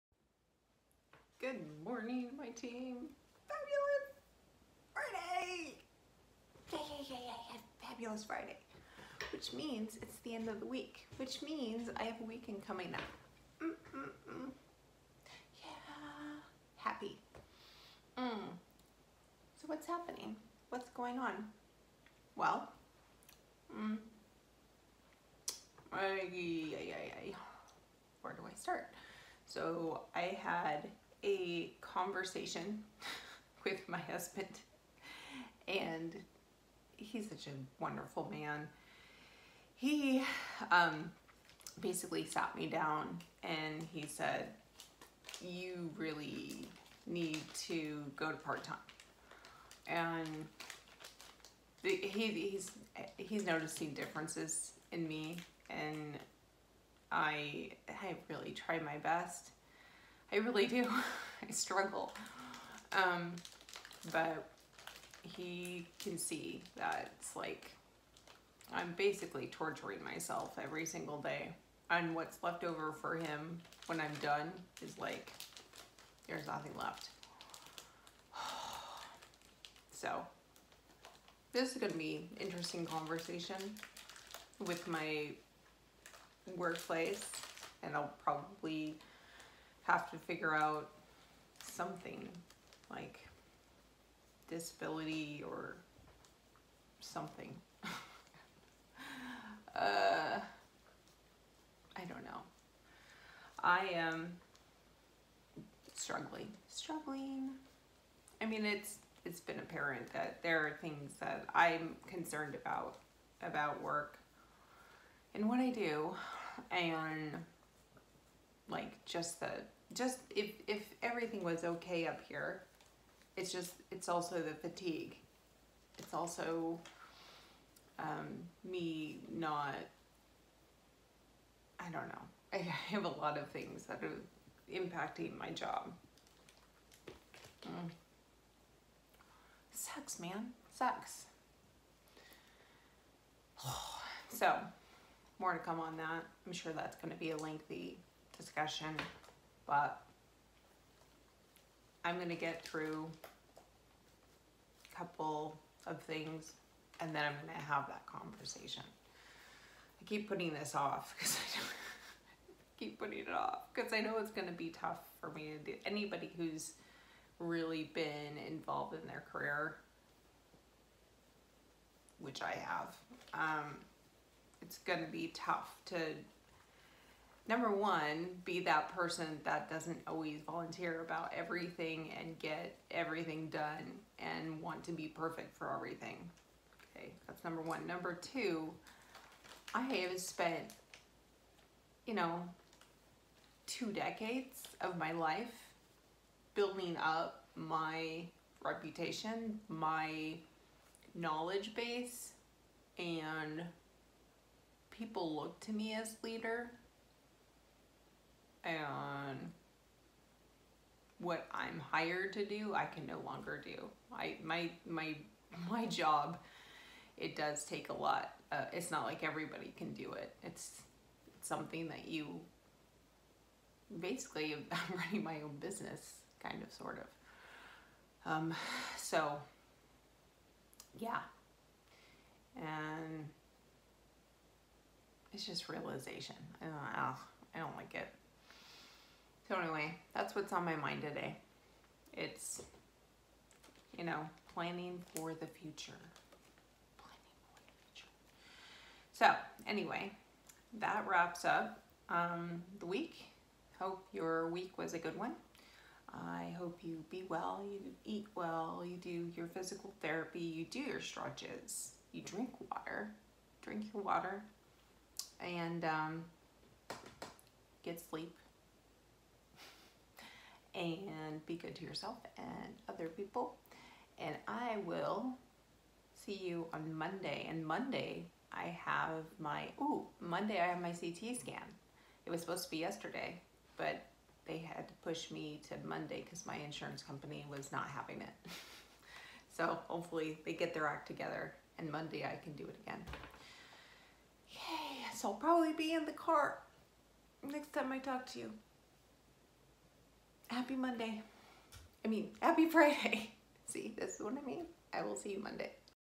The Talk
Last night the hubby and I chatted about working...specifically me working.  Ugh I hate this topic but it’s a necessary discussion.